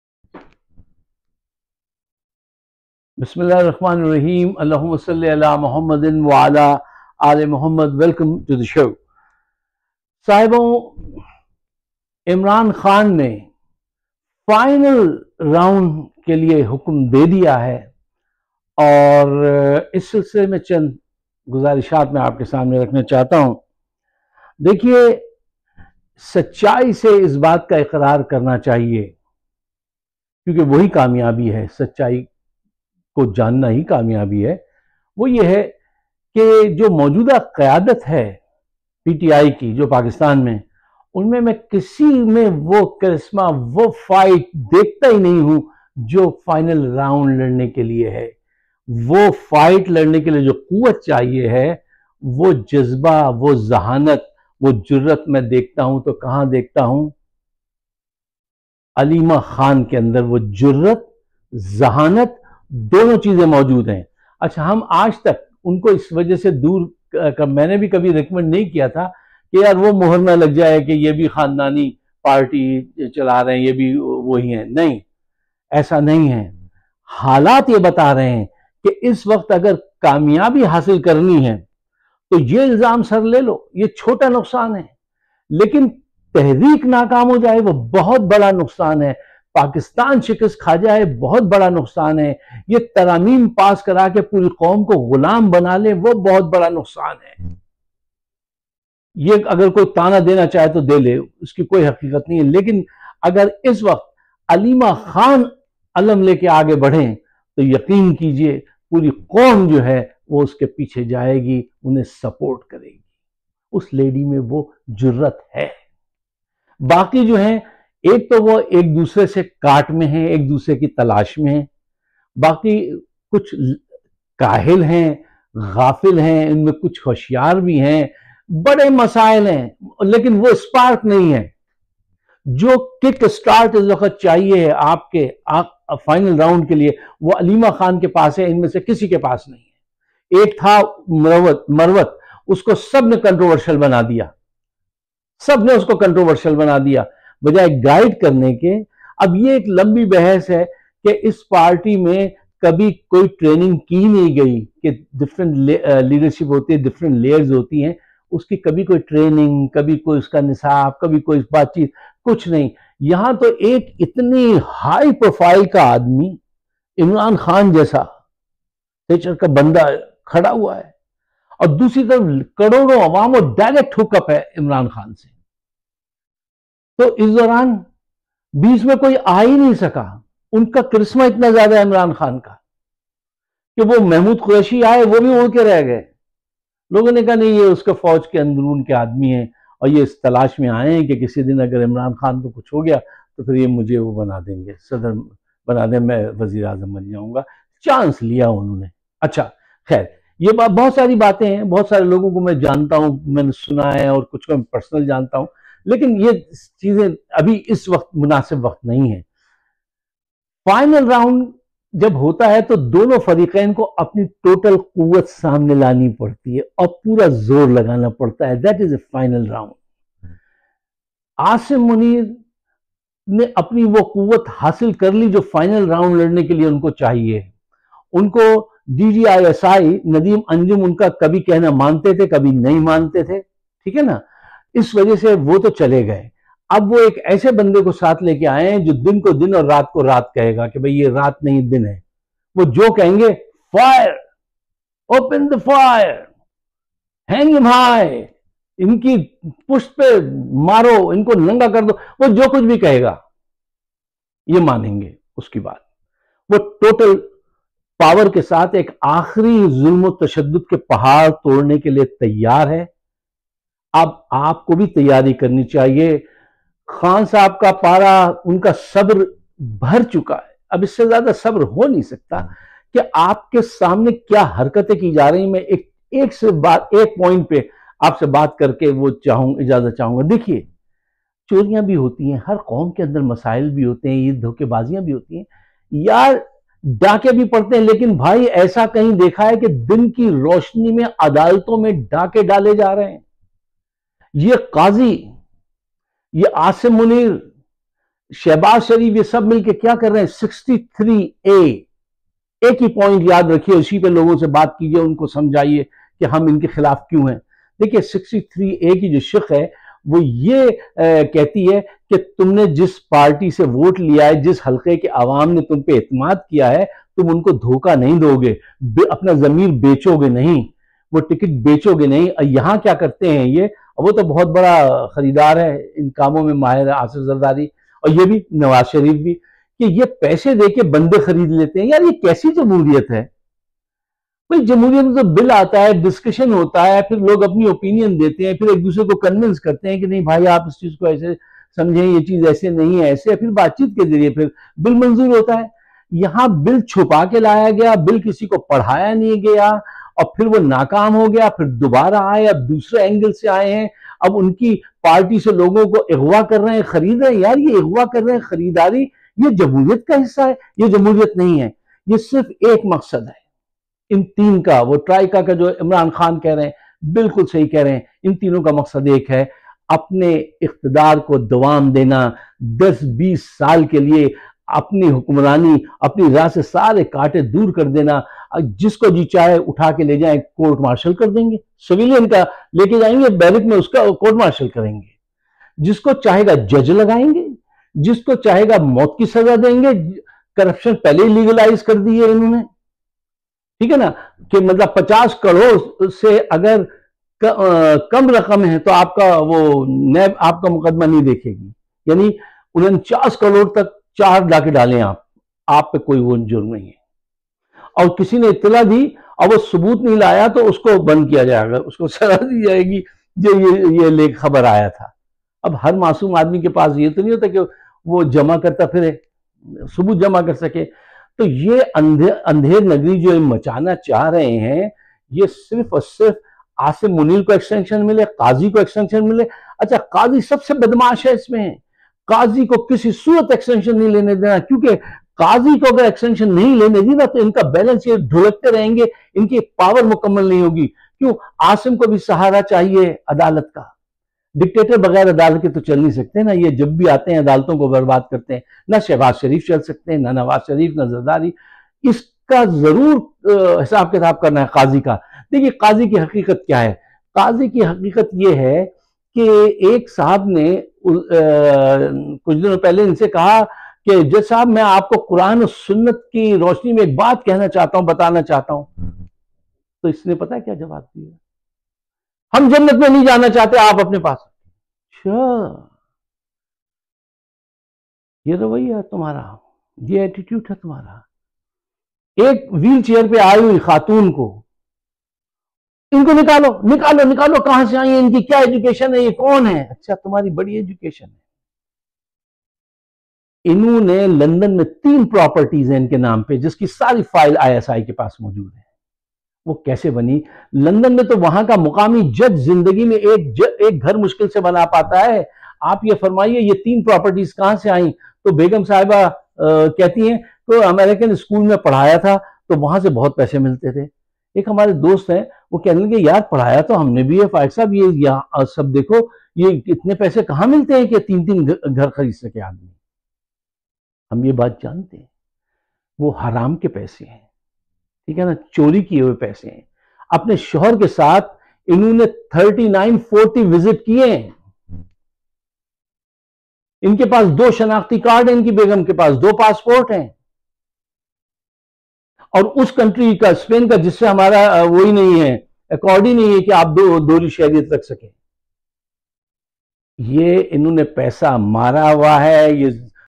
عاصم منیر بٹرا ہاتھ ڈالنے کیلے تیار۔PTI کی قیادت ذہین بہادر علیمہ خان نے نہ کی تو ہار جایں گے
پاکستان کی سیاسی صورت حال ابتری کی طرف جارہی ہے۔آئنی ترامیم جمھوریت کا گلہ گھونٹ دیں گیPolitical situation is getting worse inpakistan,Democracy is in danger